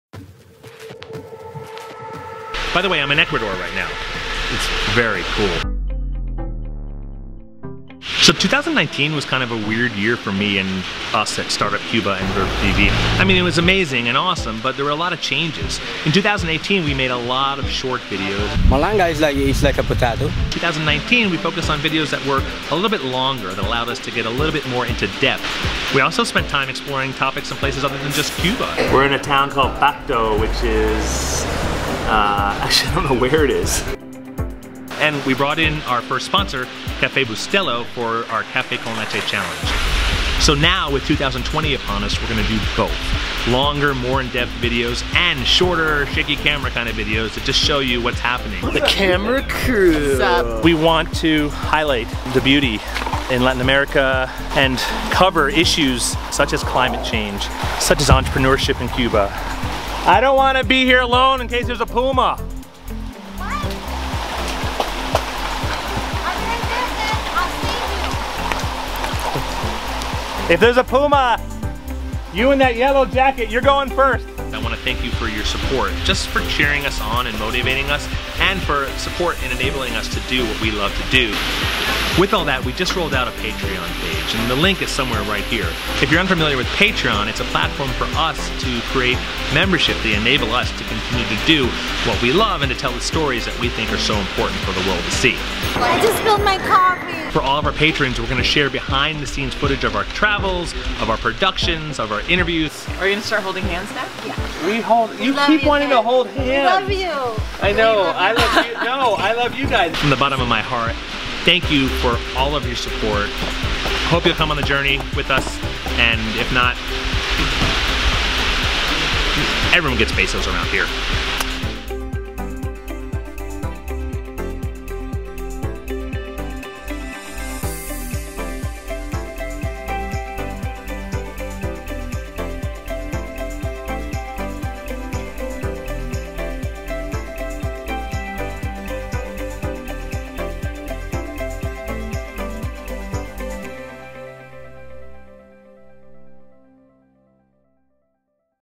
2020: Startup Cuba on Patreon
In 2020 Startup Cuba and VERV.tv are going to do both, make longer(ish) format videos and shorter ones.  We can do this now, thanks to Patreon.